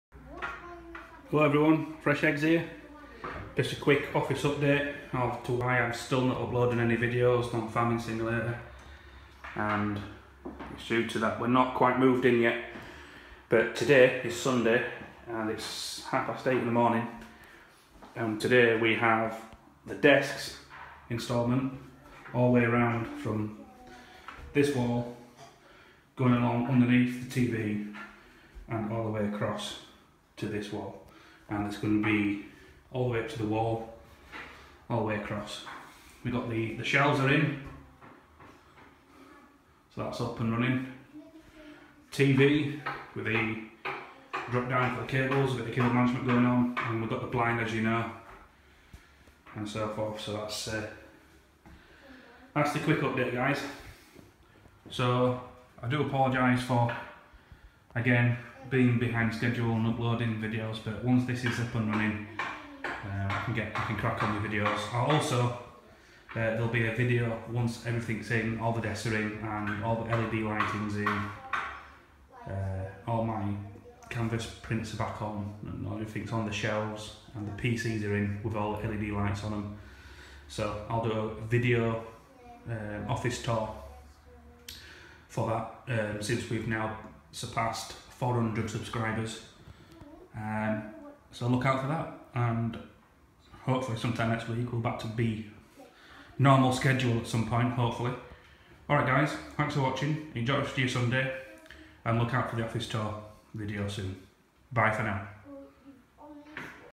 Office update (Sunday)
It's Sunday and its move in day once the desks are installed, keep an eye out for the office tour video sometime next week. Can't wait to start farming again :)